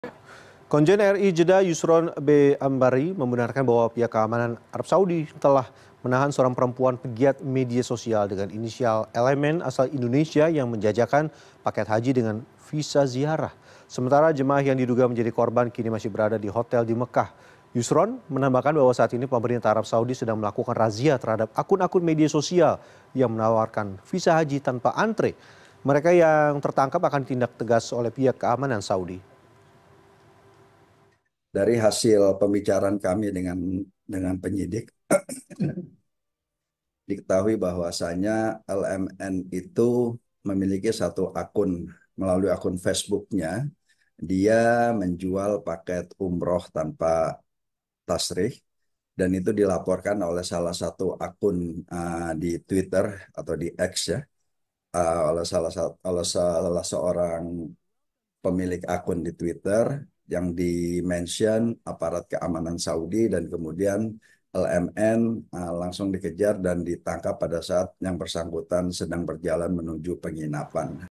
Jual Paket Haji Visa Ziarah, Pegiat Medsos Ditangkap
Pemerintah Arab Saudi telah melarang pengguna visa ziarah untuk berhaji.